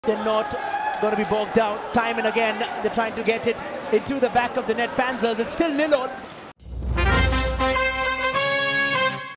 Jalal Kiani is MVP once again! | Premier Handball League
Jalal Kiani of Maharashtra Ironmen continued with his stellar performances to win another MVP award.
This was a second straight win for the Ironmen as they moved top of the table.